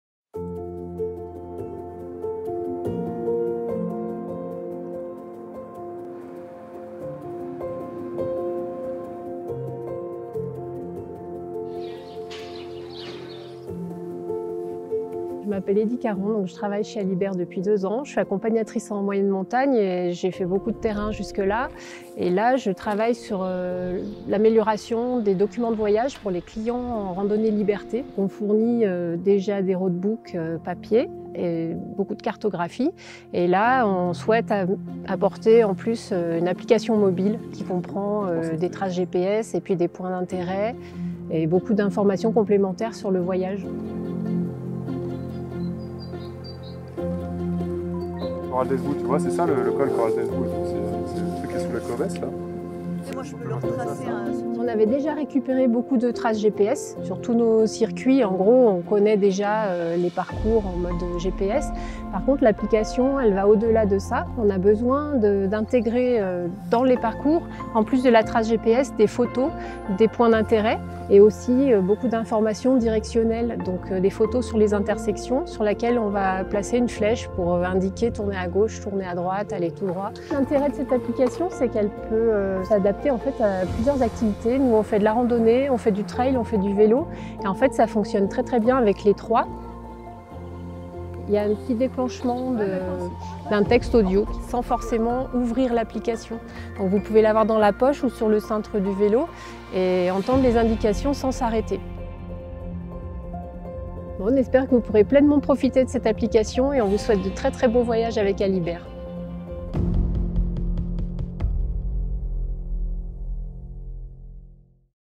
Allibert Trekking - Application "Mon Roadbook"
Tournage Mars 2019
Client : Allibert Trekking

Découverte de l'application "My Roadbook" avec un angle "behind the scenes"